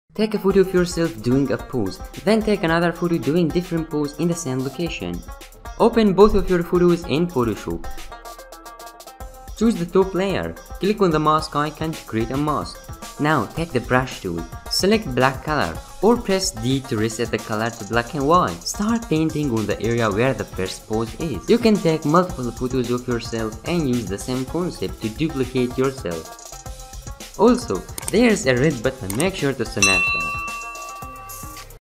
BEST WAY TO CLONE TWO OF YOURSELF IN PHOTO | PHOTOSHOP TUTORIAL
How to clone yourself in a photo! In this video, I show all the step by step editing processes behind cloning yourself in photo!

Including a beginner editing tutorial in Photoshop
this short video editing tutorial shows you how to make duplicates of yourself in a photo.